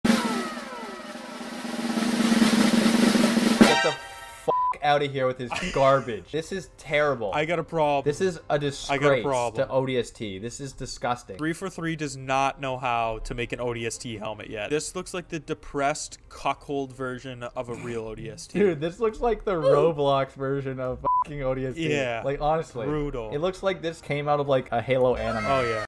Why 343 Should NEVER Make an ODST Halo Game
Well 343 hasn't made a good looking ODST helmet yet IMO, BUT the leaked images of Halo Infinite's ODST helmets look sexy